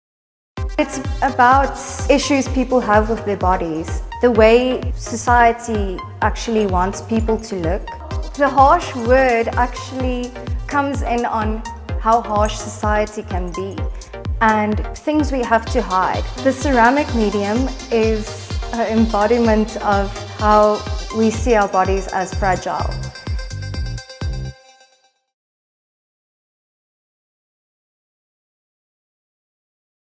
SasolNewSignatures 2017| Merit Winner| Gretchen Crots
Artists Description

The ceramic book represents the severity of the
human body and anatomy in a less severe way.
The artworks play on the futility of the human
existence and how a subject of such serious
matter can seem of little importance when looked
at from a subjective point of view.
The book depicts how the human body can be
seen and interpreted as an object in our day and
age. With references to medical and scientific
facts about the human body, the essence of
“human” comes off as irrelevant and sometimes,
even as a joke.
The goal of the book is the realisation of the
human and its rather small impact and role in
society and on earth, the human is reminded that
it is replaceable, disposable and unimportant -
just like an object being made fun of.